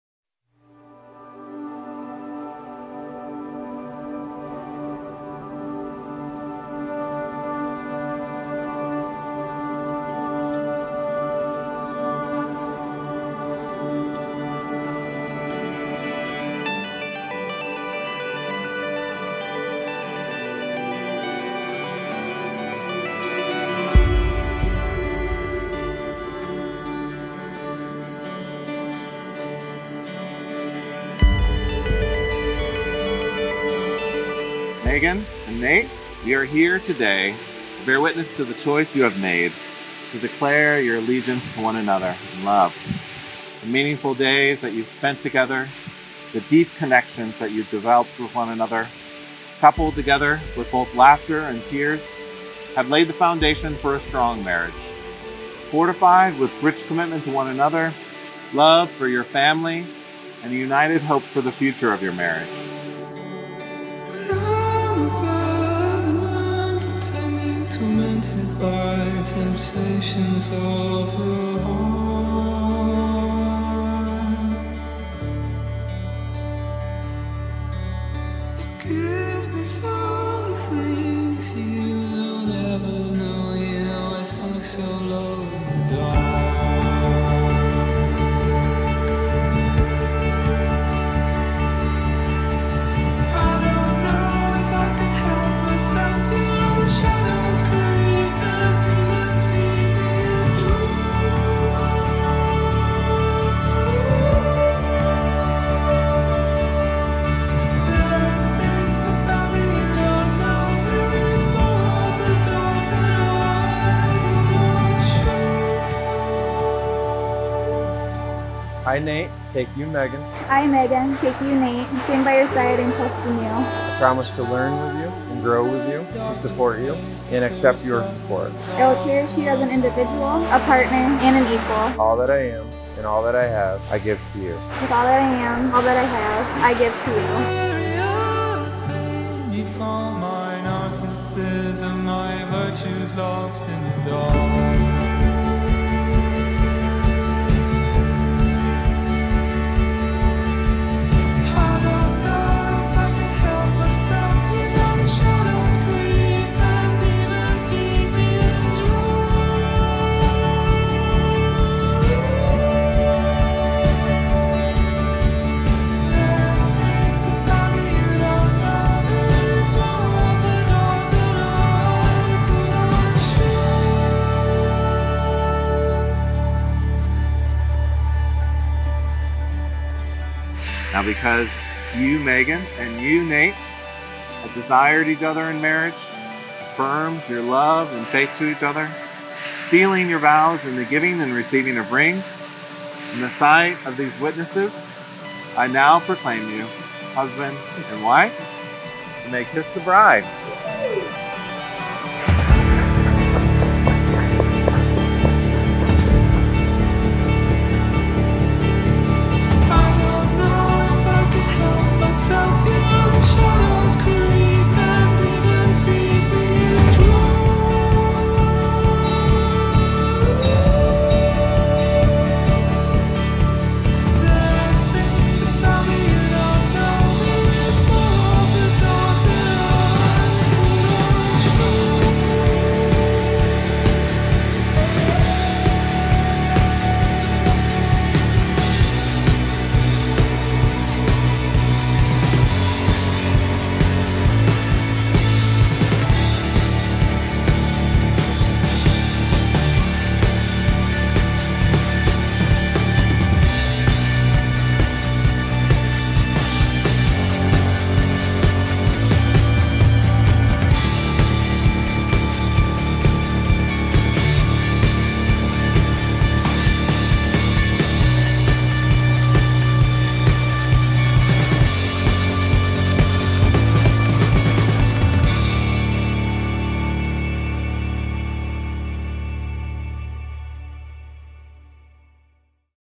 Waterfall Elopement in Asheville NC
Stunning waterfall springtime elopement with Elope Outdoors ~ Asheville